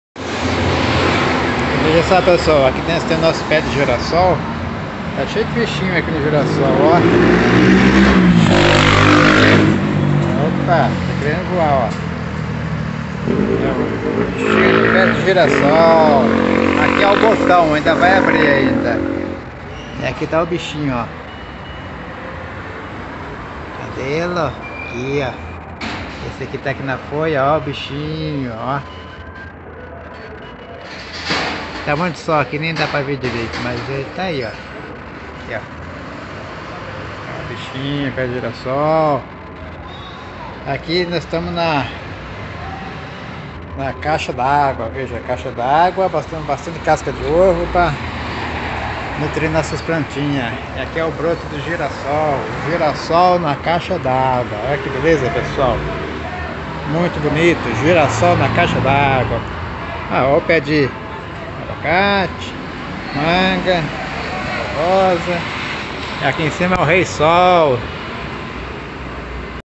pé de girassol 🌻🌻🌻 na caixa d'água falta florir.
jardim na laje projeto girassol ainda não floriu plantas na caixa d'água jardim na laje projeto.